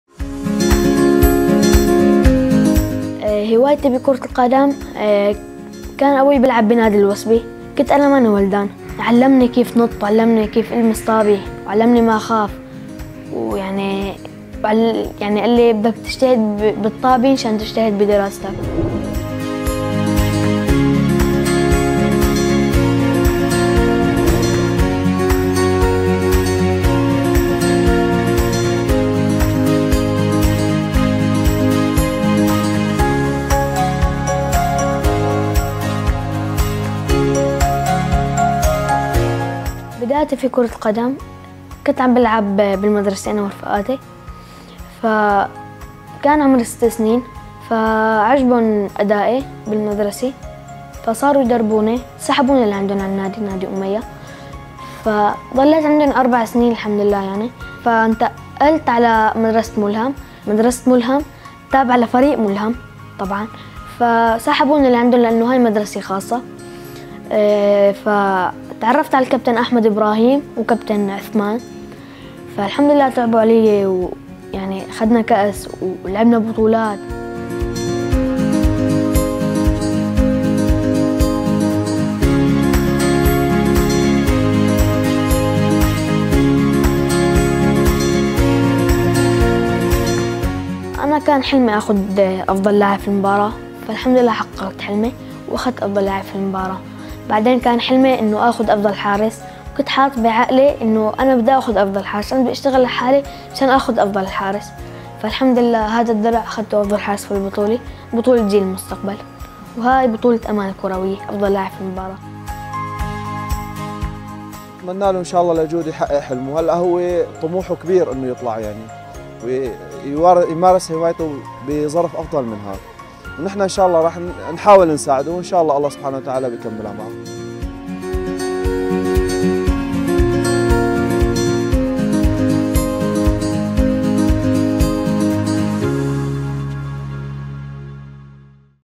جود الكردي طفل فاز بكل بطولات كرة القدم التي شارك فيها على مستوى الشمال السوري
🎥 راميا الأخرس